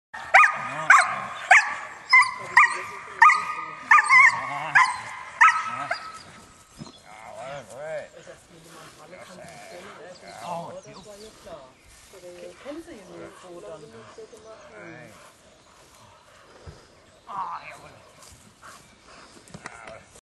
Zwinger von Himmel German Shepherd Training Protection West German Working Line German Shepherd
Young West German Shepherd ( Older pup ) Starting in Bite Work Training. Sable German Shepherd. West German Working Line German Shepherd.

Regards,
TEAM ZWINGER von HIMMEL